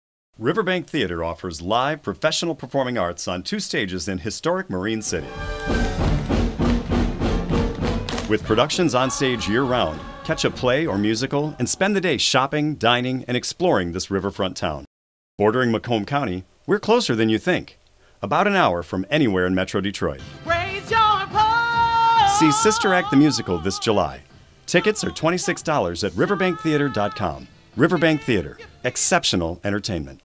Riverbank Theatre...we're closer than you think
Riverbank Theatre offers live, professional performing arts on two stages in historic Marine City. With productions on stage year round, catch a play or musical and spend the day shopping, dining, and exploring this riverfront town. Bordering Macomb County - we’re closer than you think – about an hour from anywhere in Metro Detroit.

Riverbank Theatre. Exceptional Entertainment.